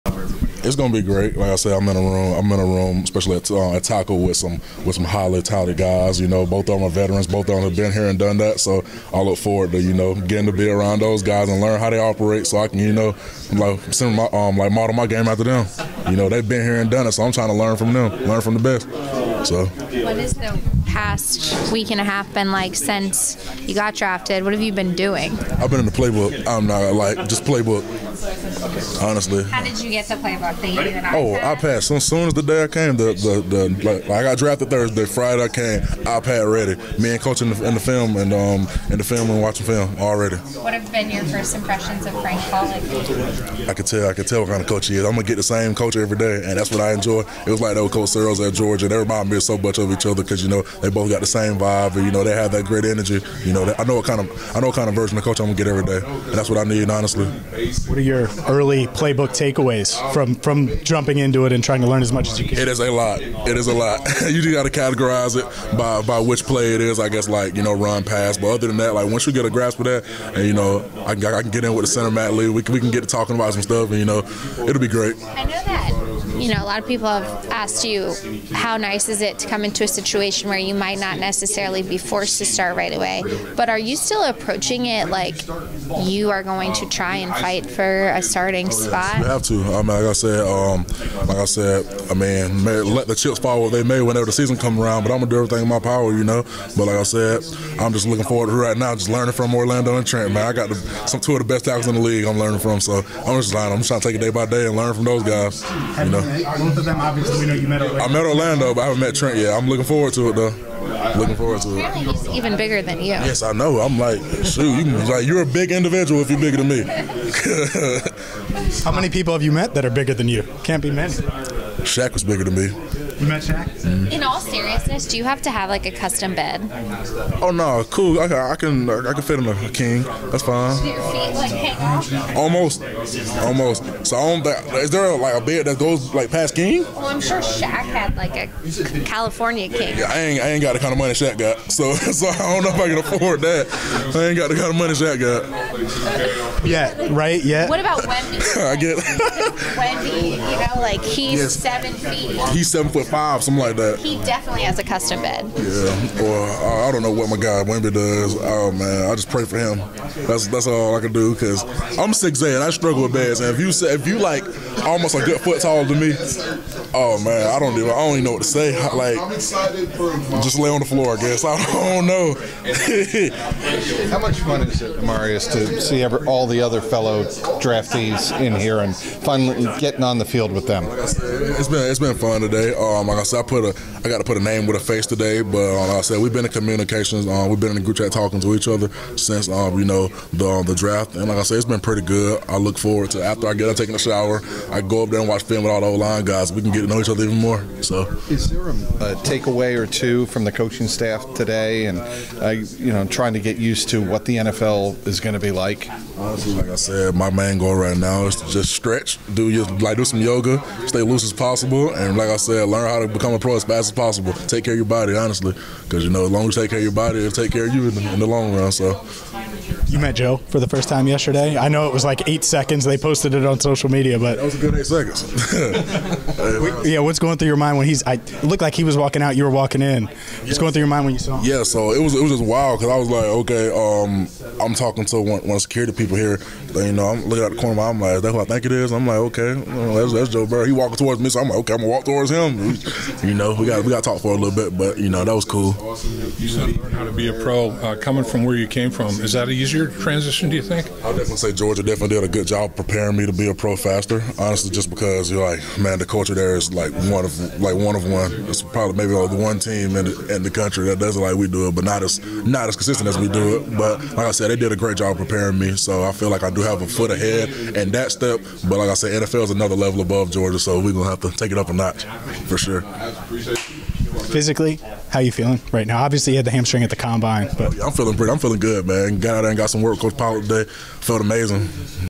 Amarius Mims on Bengals Rookie Minicamp, Meeting Joe Burrow and More
Cincinnati Bengals first round pick Amarius Mims discussed rookie minicamp, what it was like meeting Joe Burrow, what it’s like working with Frank Pollack, his health and much more!

Andrew Miller @andrewfoxmiller on Twitter, Instagram.